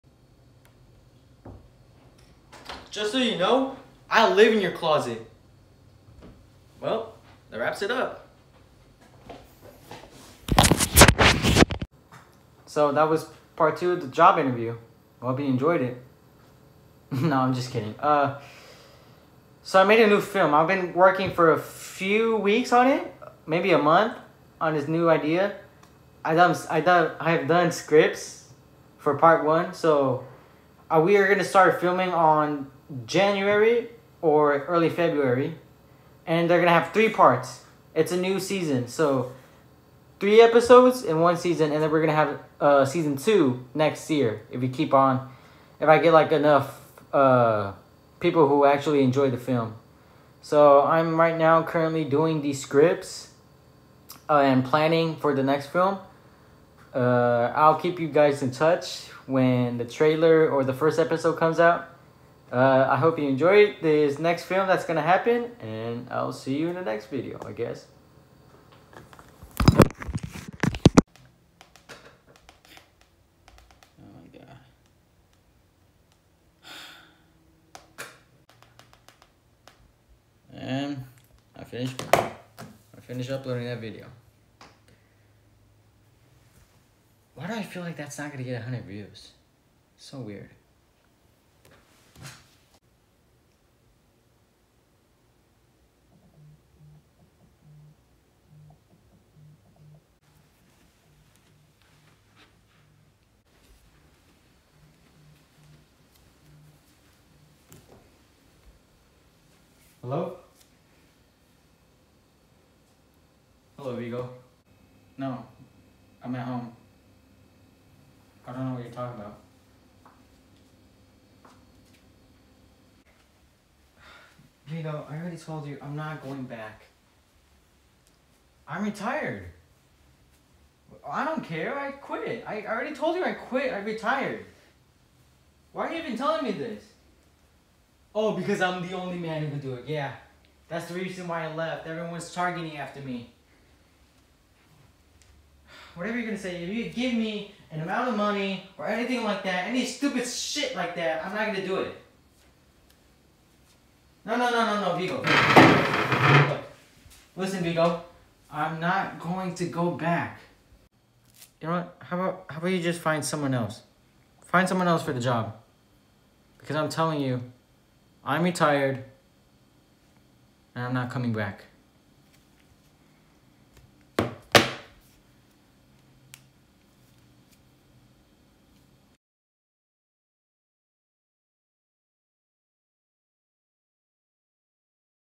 The Job Interview (Teaser Trailer) - 2021
Filming begins in January.
Hope you enjoyed the trailer and I’ll let you all know when the first episode drops in my channel.

Thank you and Happy New Years!
Stay Safe ;)